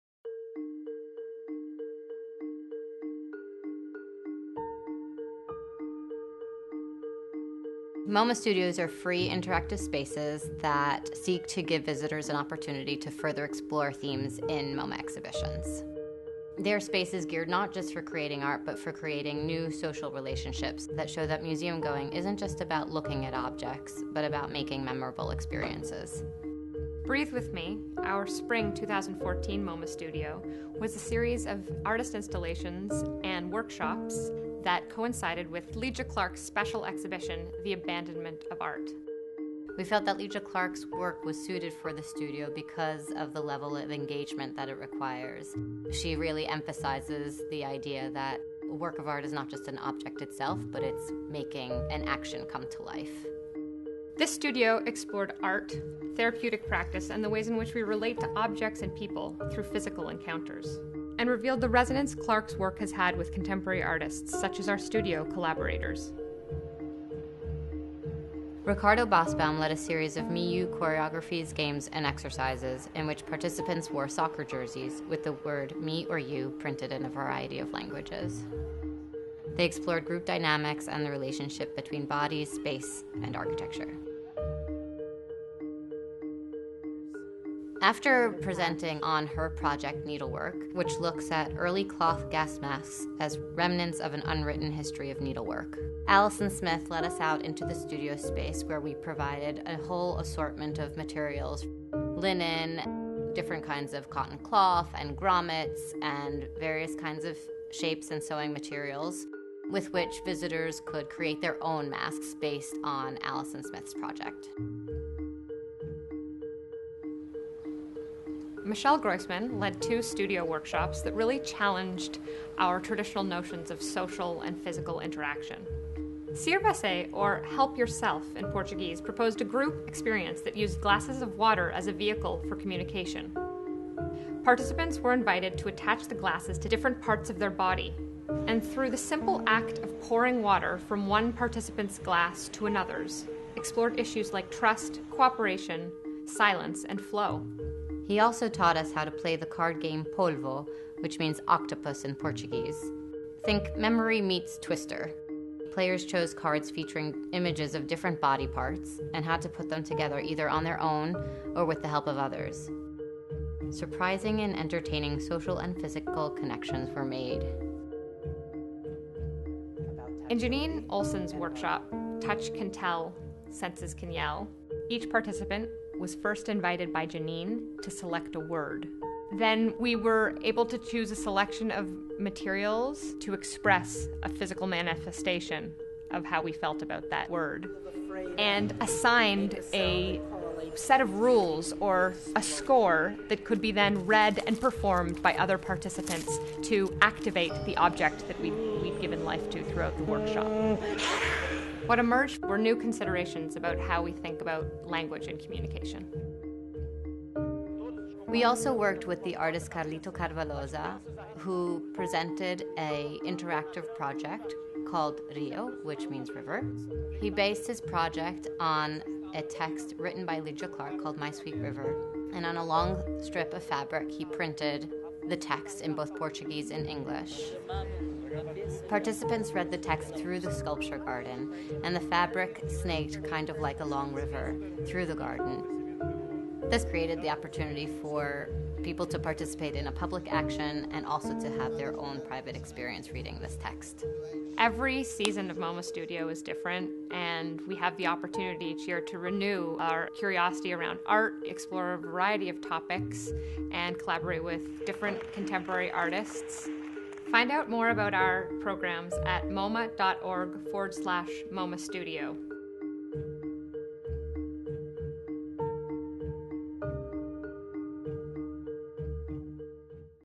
MoMA Studio: Breathe with Me
Organized in conjunction with the exhibition Lygia Clark: The Abandonment of Art, 1948–1988, MoMA Studio: Breathe with Me is an interactive space that explores the intersections between art, therapeutic practice, and the ways in which we relate to objects and people through physical encounters. Taking Lygia Clark’s art as a reference point, the Studio presents a series of drop-in programs, participatory experiences, and artist-led workshops that reveal the profound resonance Clark’s work has had with contemporary artists. Collaborating artists include Ricardo Basbaum, Carlito Carvalhosa, Stephanie Diamond and Tamara Vanderwal, Michel Groisman, Jeanine Oleson, and Allison Smith.